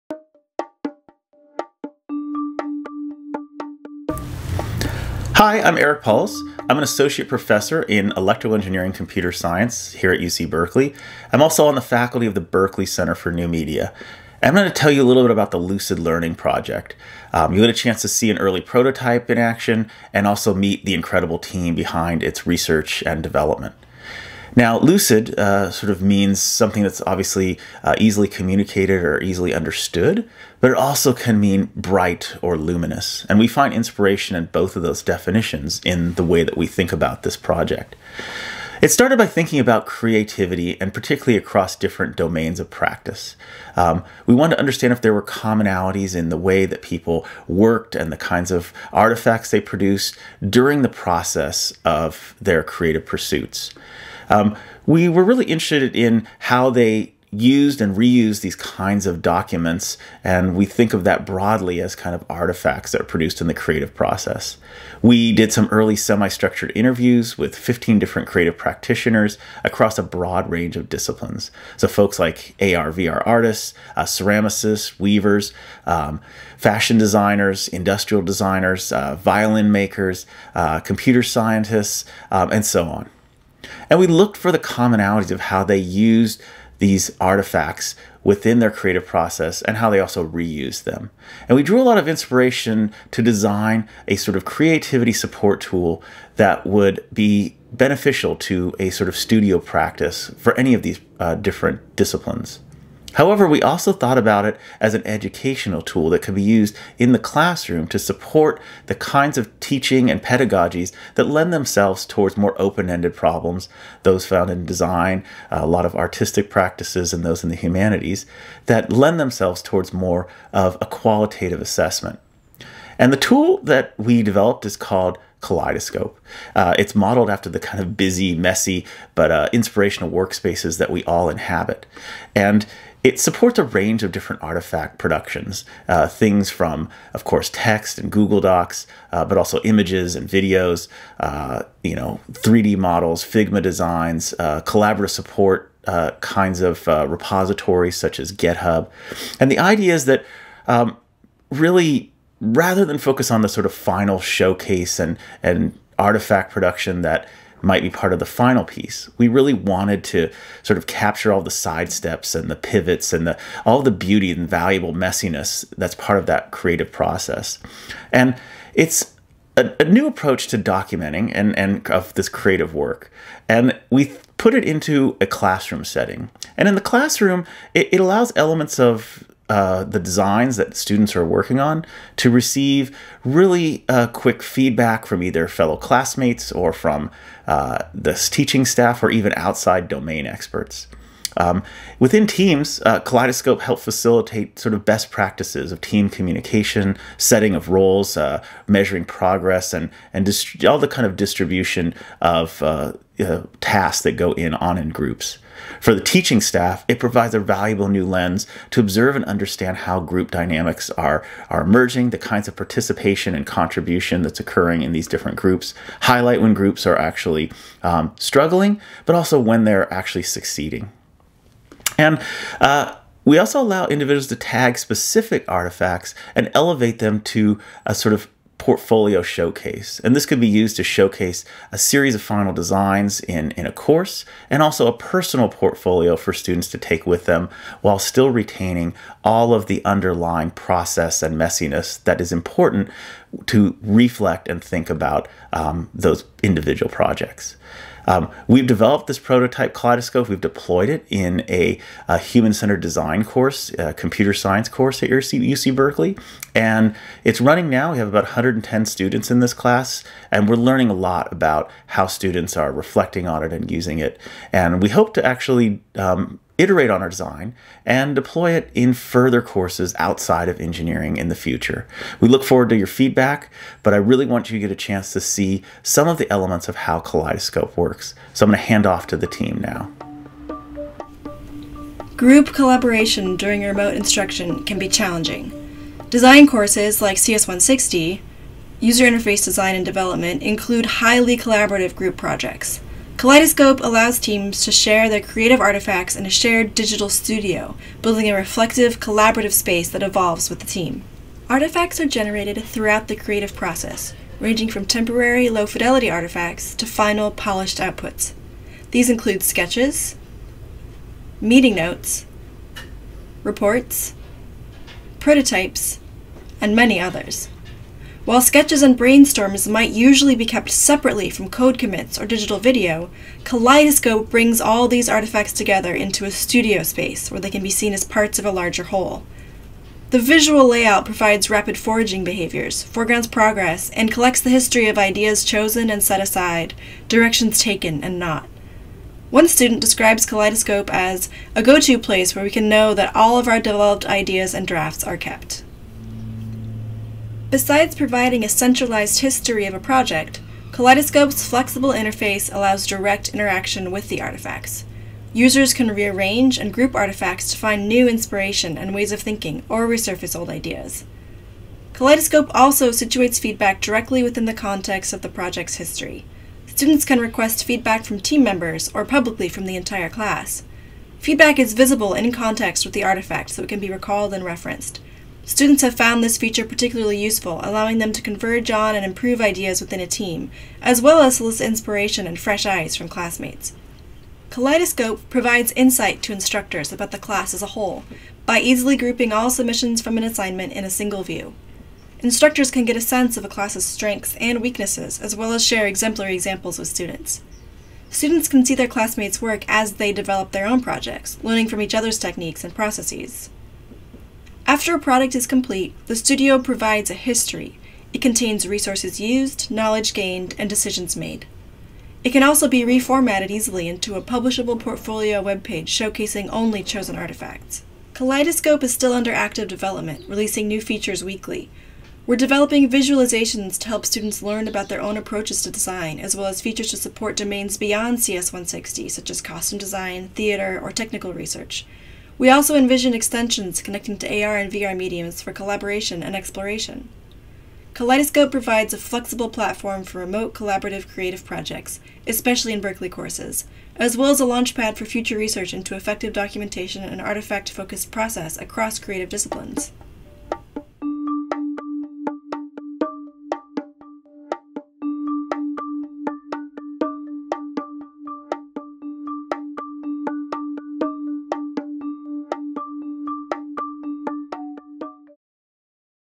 FTIF20-11 Paulos - Lucid Learning Kaleidoscope (2020 UC Berkeley Fall Technology Innovation Forum)
Lucid Learning is a tool to help students practice collaboration early in their careers. The proposed technology aims to foster a process for growing ideas, debating, critiquing, iterating on qualitative projects that span across domains of expertise that universities typically produce in students.